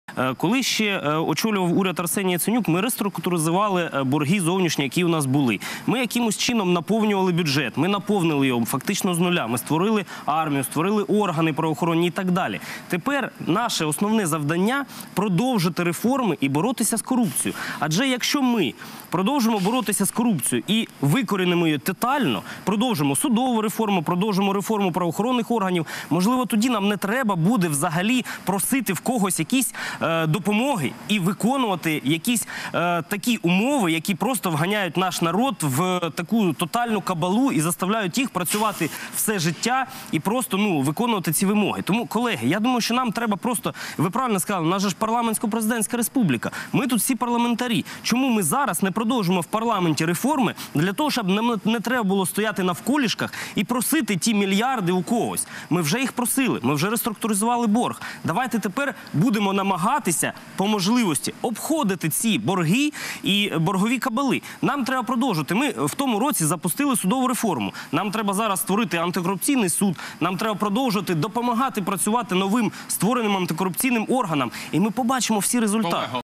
Успішна боротьба з корупцією допоможе уникнути зовнішніх запозичень, - Андрій Помазанов
Успішна боротьба з корупцією допоможе Україні уникнути подальших запозичень у міжнародних фінансових організацій.

Про це заявив народний депутат фракції «Народний фронт» Андрій Помазанов у вівторок в ефірі «5 каналу».

«Коли уряд очолював Арсеній Яценюк, ми реструктурували зовнішні борги, наповнили бюджет фактично з нуля, створили армію, правоохоронні органи, і тепер наше основне завдання – продовжити реформи й боротися з корупцією», - сказав народний депутат.

«Адже якщо ми викорінимо корупцію за допомогою реформи судових та правоохоронних органів, нам, можливо, взагалі не доведеться просити допомоги ззовні», - додав він.

«Минулого року ми запустили судову реформу. Тепер нам треба створити антикорупційний суд, треба допомогти працювати новим антикорупційним органам. І ми побачимо, який буде результат», - підсумував Андрій Помазанов.

Приєднуйся!